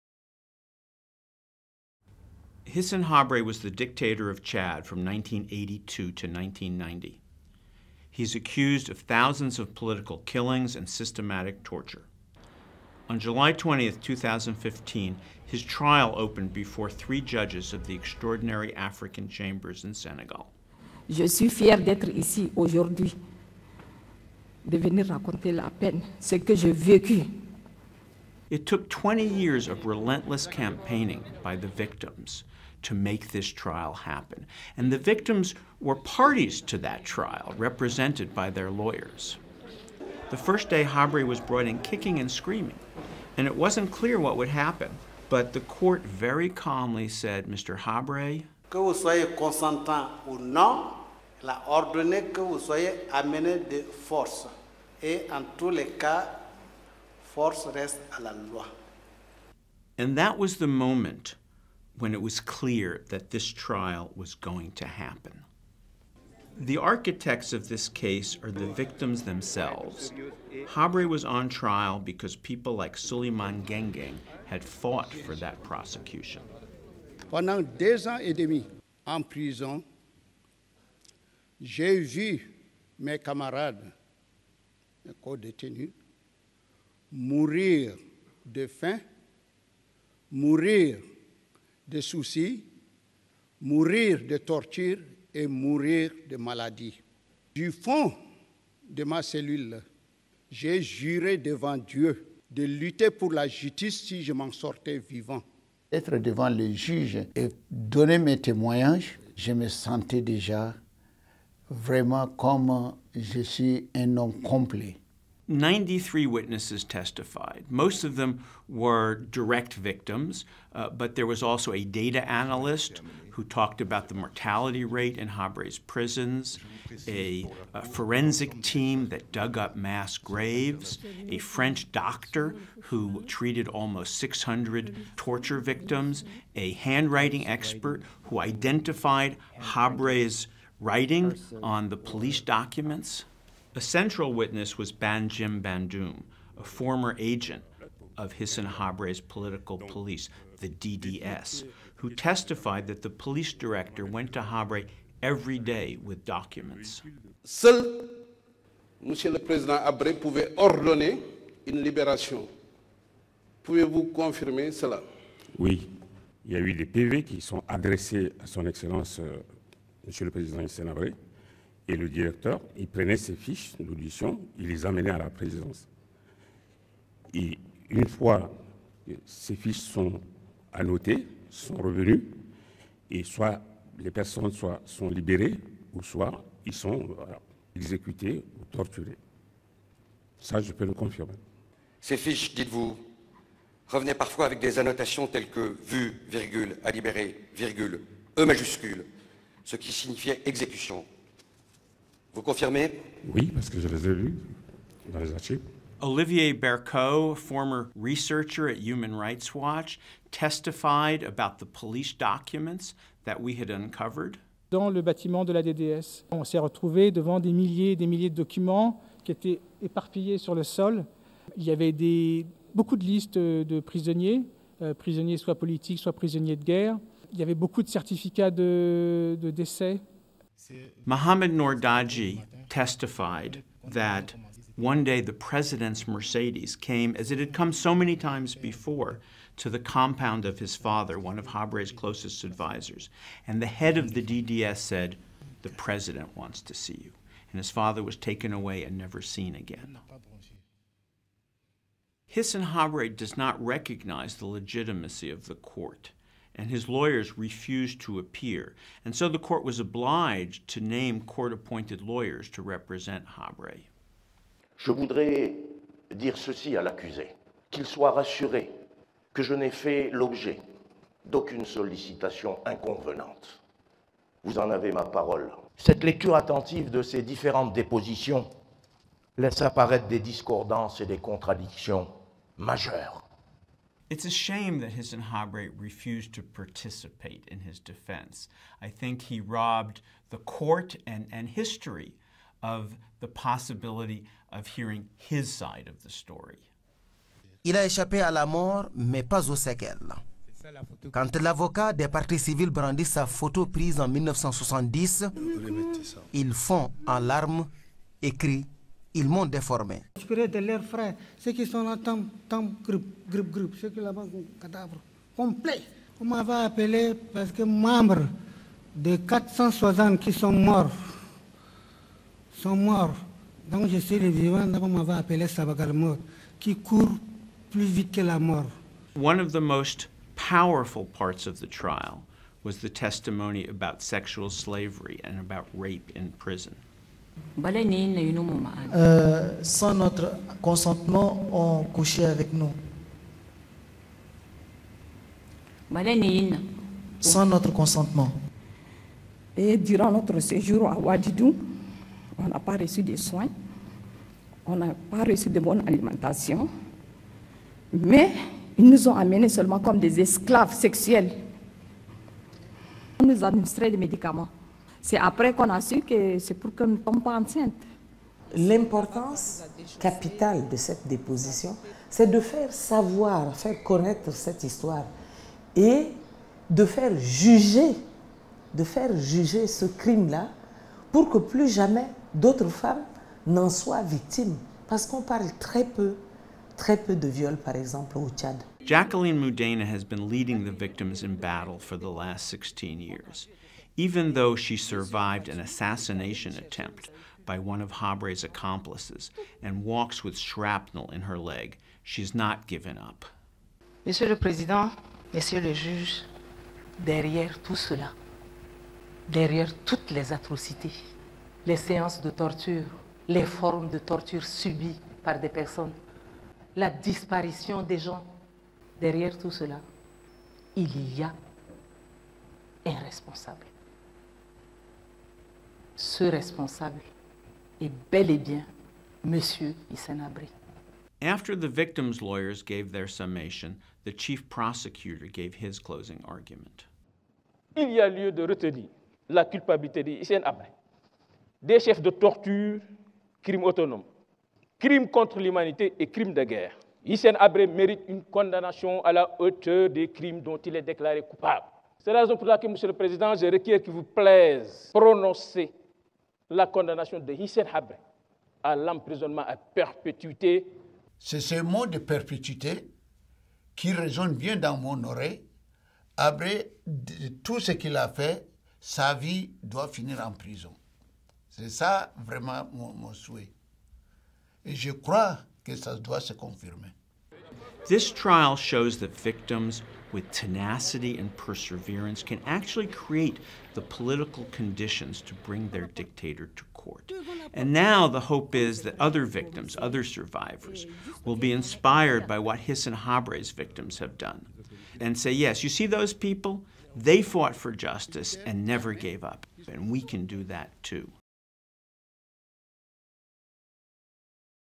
Hissène Habré: Scenes from an historic trial
The former Chadian dictator Hissène Habré's trial is taking place in Senegal. Habré’s trial, which began on July 20, 2015, was the first in the world in which the courts of one country prosecuted the former ruler of another for alleged human rights crimes. Witness hearings ended in December  after 52 days of trial, and were followed by the parties' summations in February. The new Human Rights Watch video highlights some of the key moments of the trial.

At the end of the trial on February 11, 2016, the court announced that the verdict would be scheduled for May 30. Habré faces charges of crimes against humanity, torture, and war crimes before the Extraordinary African Chambers, which is part of the Senegalese court system.